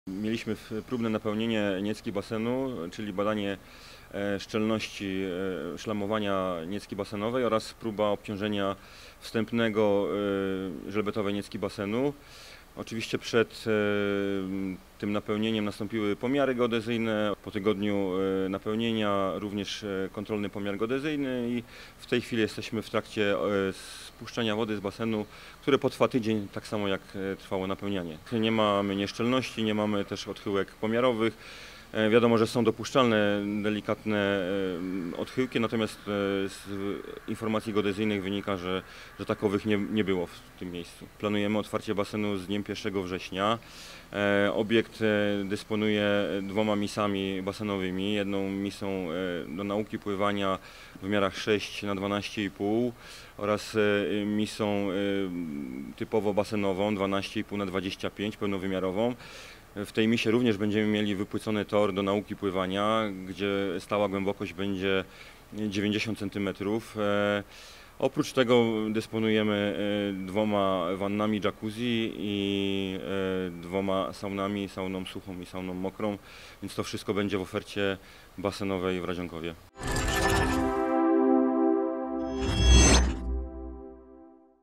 Basen w budowie - Radzionków. Próbne napełnienie głównej niecki
W Radzionkowie trwa budowa krytego basenu. Odbyło się próbne napełnienie głównej niecki pływalni. O jego wynikach mówi Grzegorz Szeremeta, zastępca burmistrza Radzionkowa.